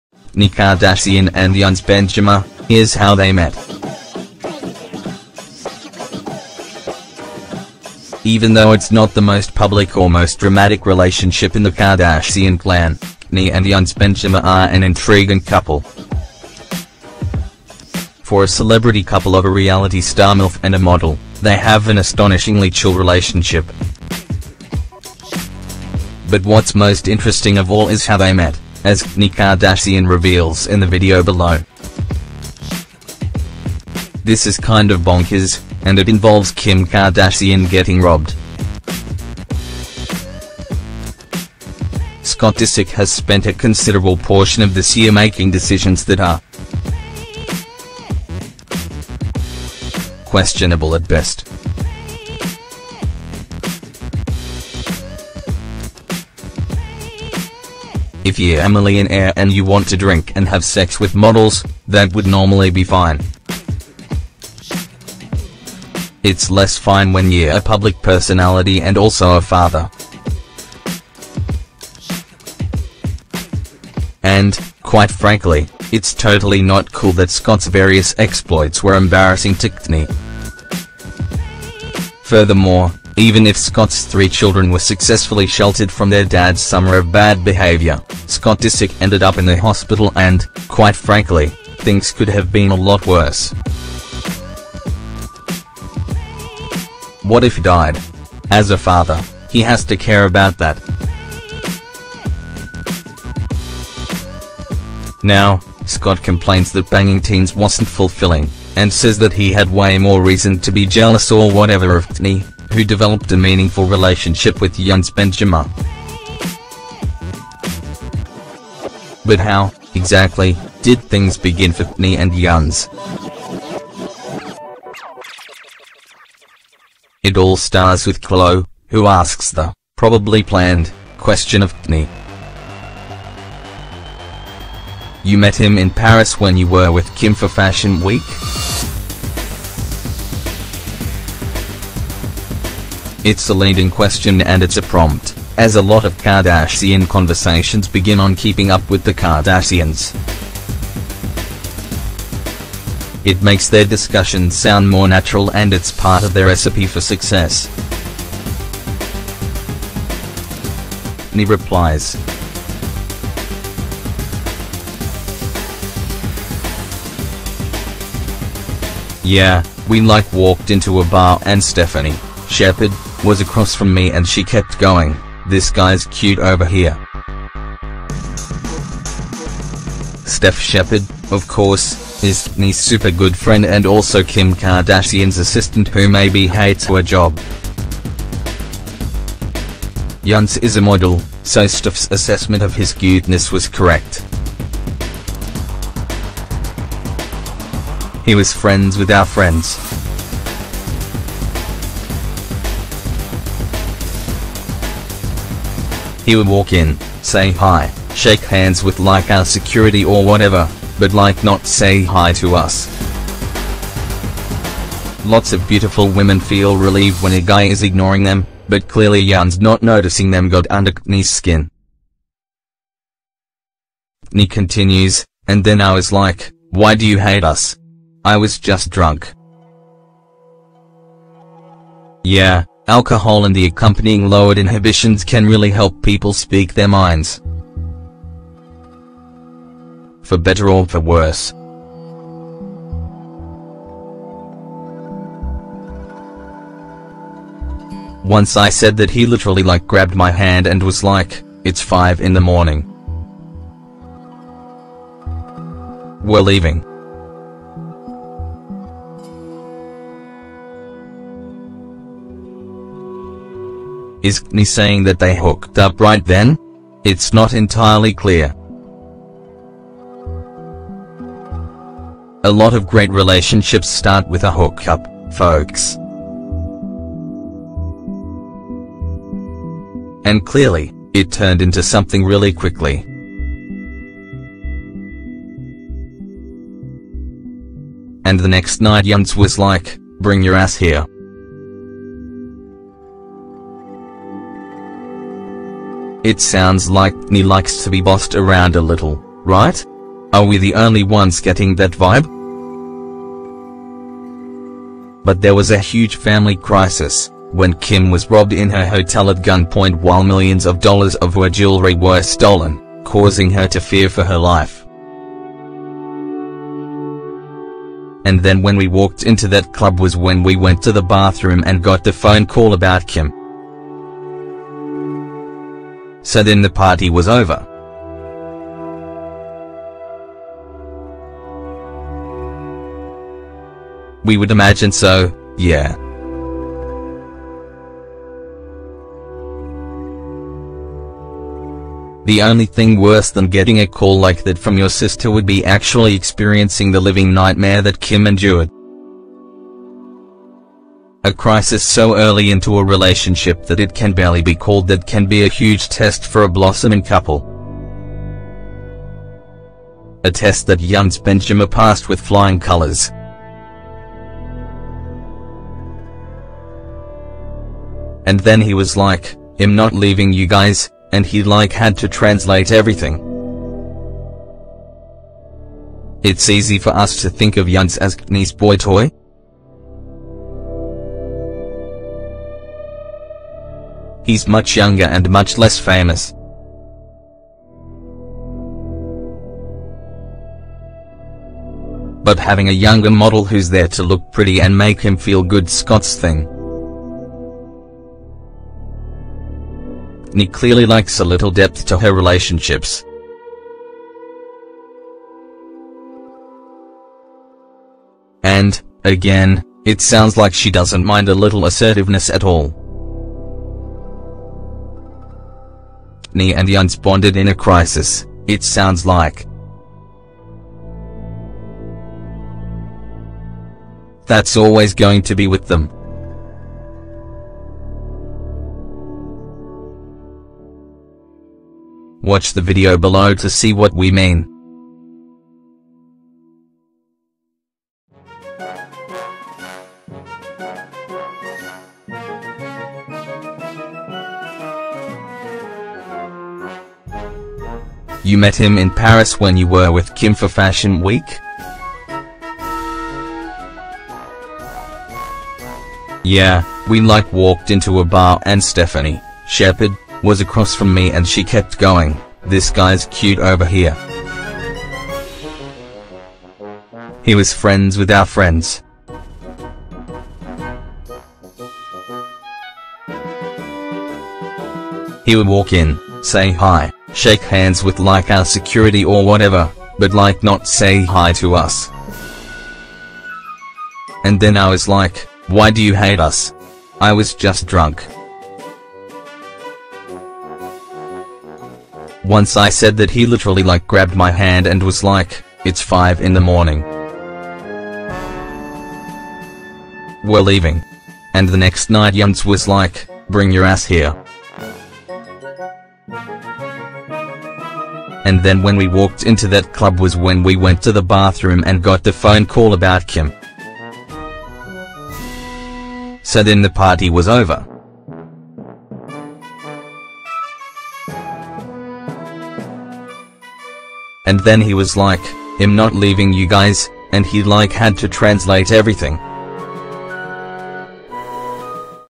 Kourtney Kardashian & Younes Bendjima: Here's How They Met!
Even though it's not the most public or most dramatic relationship in the Kardashian clan, Kourtney and Younes Bendjima are an...